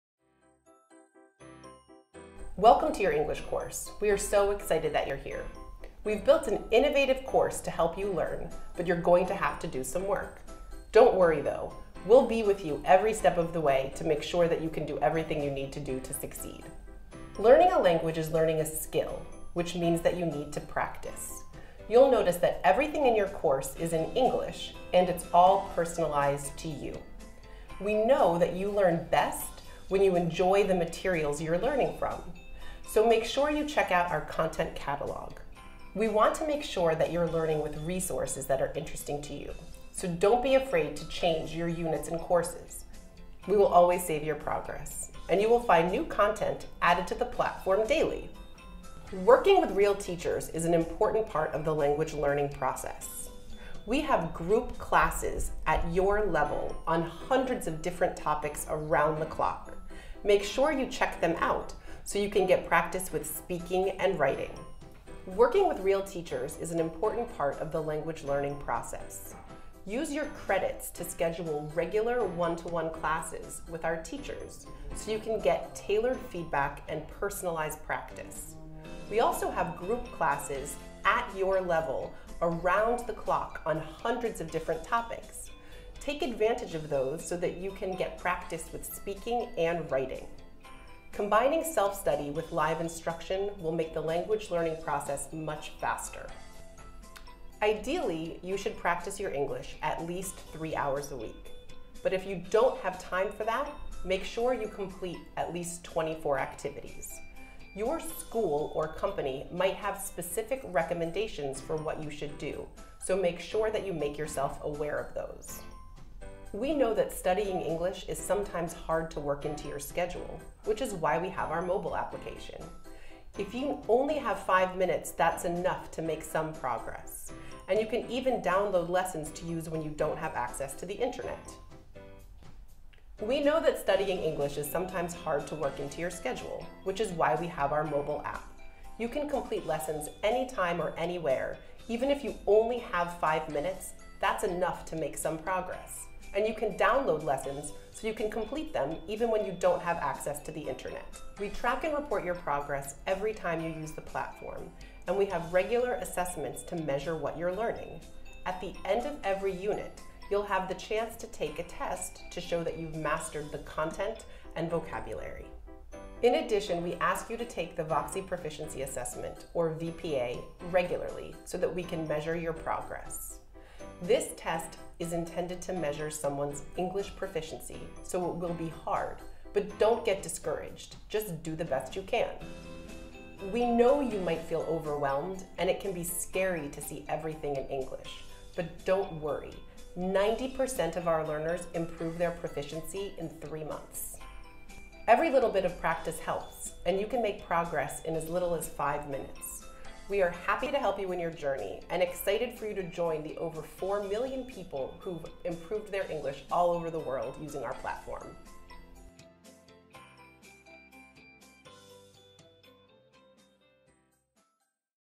Welcome to Voxy English
Join one of the top English learning platforms in the world.

Here's a quick overview of Voxy and how it helps you learn English from the real world!

Study English now 24/7 Courses wherever and whenever you want!